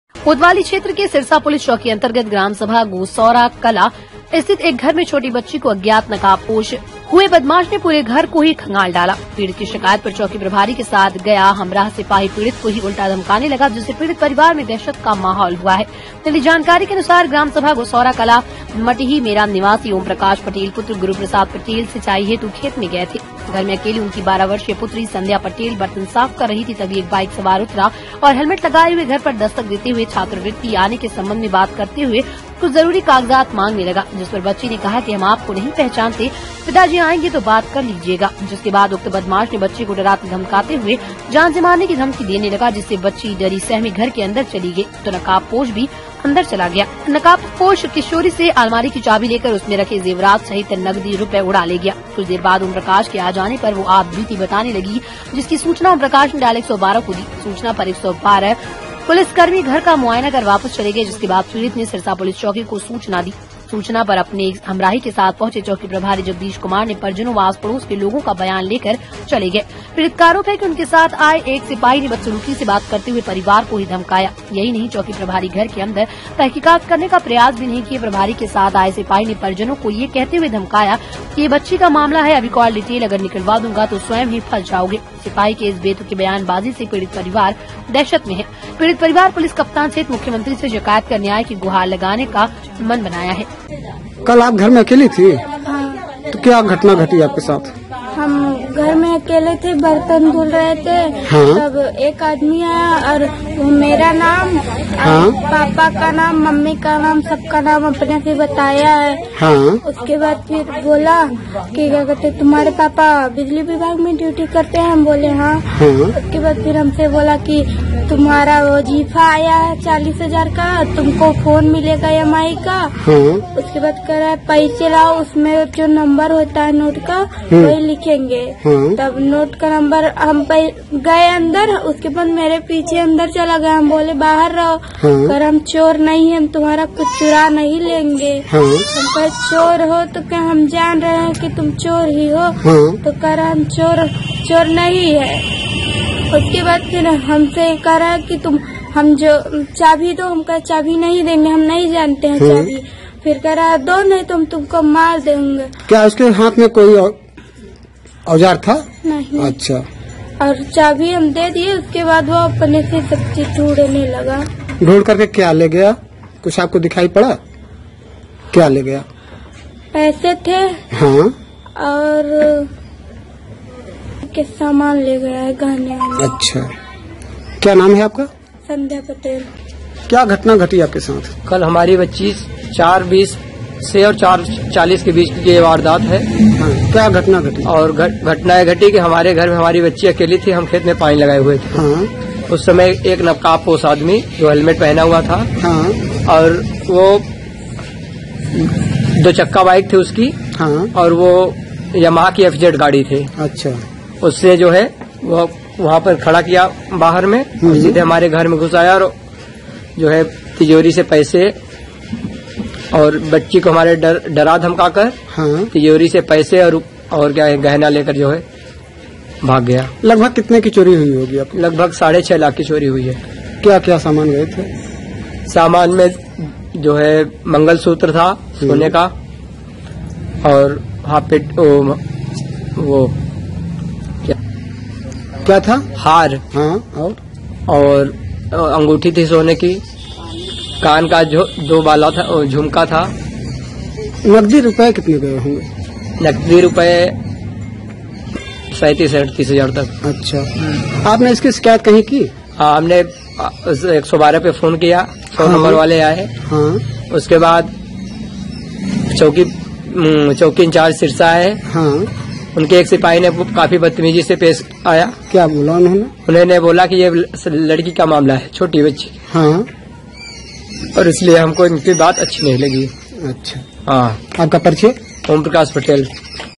बदमाश ने दिनदहाड़े किशोरी को धमकाकर लाखों का माल किया पार...
हमसे जुडने के लिए इन लिंक्स पर फॉलो करें:
News Paper
कोतवाली क्षेत्र के सिरसा पुलिस चौकी अंतर्गत ग्राम सभा गोसौरा कला (मटिही) स्थित एक घर में छोटे बच्ची को अज्ञात नकाबपोश (हेलमेट लगाए) हुए बदमाश ने पूरे घर को ही खंगाल डाला। पीड़ित की शिकायत पर चौकी प्रभारी के साथ गया हमराह सिपाही पीड़ित को ही उल्टा धमकाने लगा जिससे पीड़ित परिवार में दहशत का माहौल बना हुआ है।
मिली जानकारी के अनुसार ग्राम सभा गोसौरा कला मटिही मेरा निवासी ओम प्रकाश पटेल पुत्र गुरु प्रसाद पटेल सिंचाई हेतु खेत में गए हुए थे। घर में अकेली उनकी 12 वर्षीय पुत्री संध्या पटेल बर्तन साफ कर रही थी। तभी एक बाइक सवार उतरा और हेलमेट लगाए हुए घर पर दश्तक देते हुए छात्रवृत्ति आने के संबंध में बात करते हुए कुछ जरूरी कागजात मांगने लगा। जिस पर बच्ची ने कहा कि हम आपको नहीं पहंचानते पिताजी आएंगे तो बात कर लीजिएगा। जिसके बाद उक्त बदमाश ने बच्ची को डराते-धमकाते हुए जान से मारने की धमकी देने लगा, जिससे बच्ची डरी सहमी घर के अंदर चली गई तो, नकाबपोश भी अंदर चला गया। प्रवेश किया। नकाबपोश किशोरी से अलमारी की चाभी लेकर उसमें रखे जेवरात सहित नगदी रुपए उड़ा ले गया। कुछ देर बाद ओमप्रकाश के आ जाने पर वह आप बीती बताने लगी। जिसकी सूचना ओमप्रकाश ने डायल 112 को दी। सूचना पर डायल 112 पुलिस कर्मी घर का मुआयना कर वापस चले गए। जिसके बाद पीड़ित ने सिरसा पुलिस चौकी को सूचना दी। सूचना पर अपने एक हमराही के साथ पहुंचे चौकी प्रभारी जगदीश कुमार ने परिजनों व आस-पड़ोस के लोगों का बयान लेकर चले गए।
पीड़ित का आरोप है कि उनके साथ आए एक सिपाही ने बदसलूकी से बात करते हुए परिवार को ही धमकाया। यही नहीं चौकी प्रभारी घर के अंदर तहकीकात करने का प्रयास भी नहीं किया। प्रभारी के साथ आए सिपाही ने परिजनों को यह कहते हुए धमकाया कि यह बच्ची का मामला है। अभी कॉल डिटेल अगर निकलवा दूंगा तो स्वयं ही फंस जाओगे। सिपाही के इस बेतुके बयानबाजी से पीड़ित परिवार दहशत में हैं। पीड़ित परिवार पुलिस कप्तान सहित मुख्यमंत्री से शिकायत कर न्याय की गुहार लगाने का मन बनाया है।